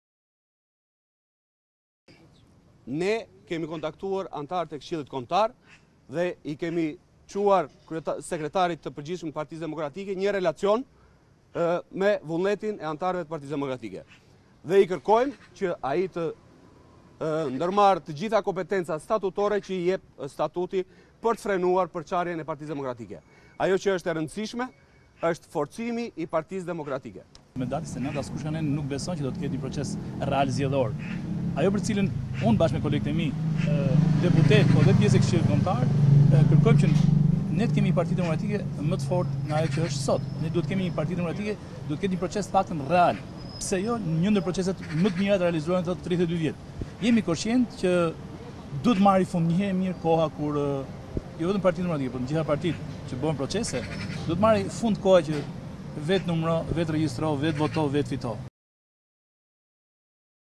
Zgjedhjet për kryetarin e ri/ Gështenja: Jo më me procese të njëanshme, sepse e shkatërron PD
Deputeti Lefter Gështenja PD nuk është pronë e askujt ndaj duhet që të bëhet një proces që e forcon dhe jo ta shkatërrojë më shumë partinë.

"Me 29 askush nga ne nuk besojnë se do të ketë një një proces real. Ne kërkojmë një proces real, dhe jemi koshientë që duhet që të marrë fund koha vetë numëro dhe vetë fito. Në këtë situatë PD-së i nevojitet një proces real", tha Gështenja.